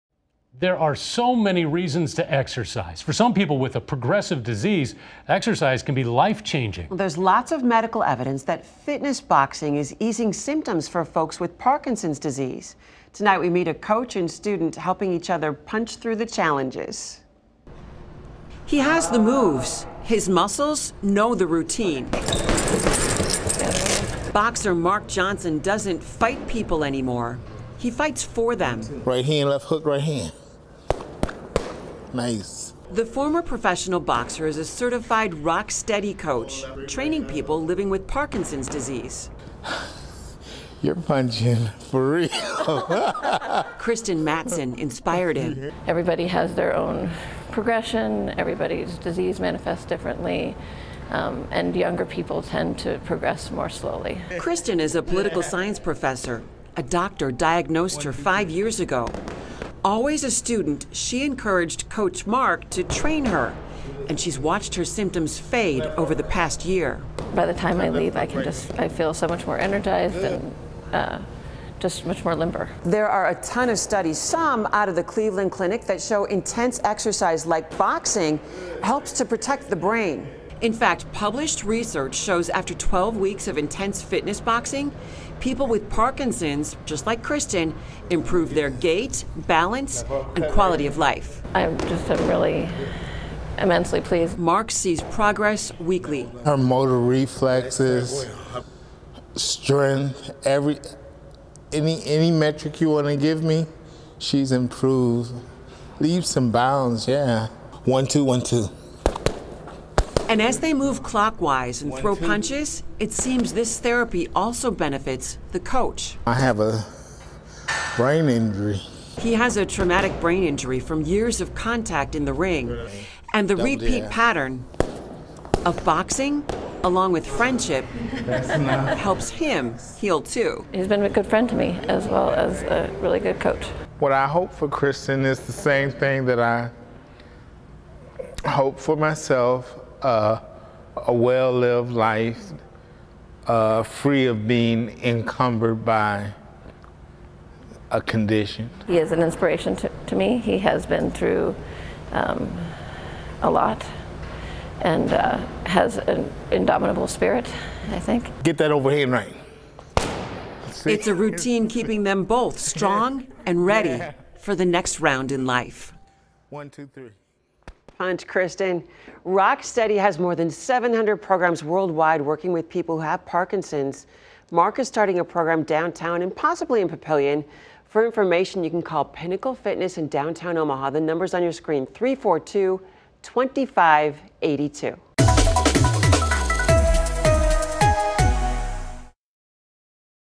Parkinson's patient finds healing, strength through boxing
Kristen Mattson has battled the disease for 5 years